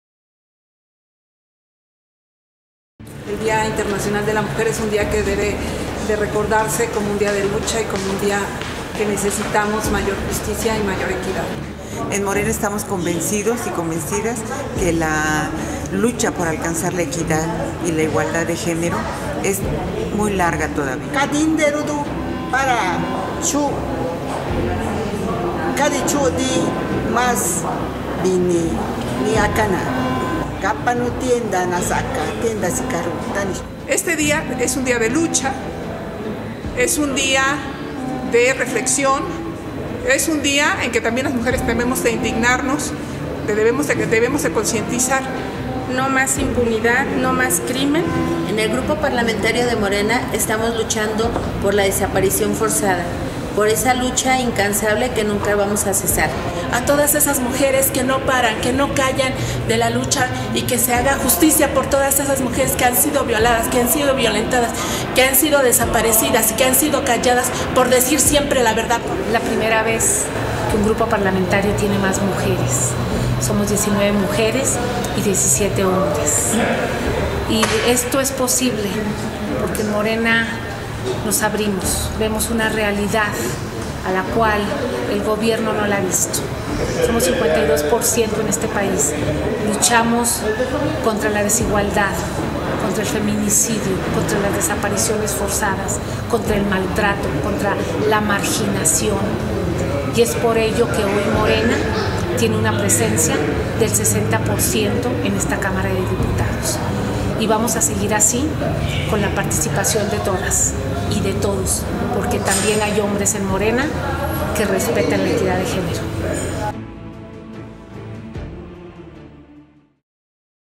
MORENA conmemora la semana del Día Internacional de la Mujer
En el marco del Día Internacional de la Mujer, MORENA, que cuenta con la mayoría de representantes femeninas en el Congreso de la Unión, conmemora esta fecha con un mensaje a todas las mujeres de México.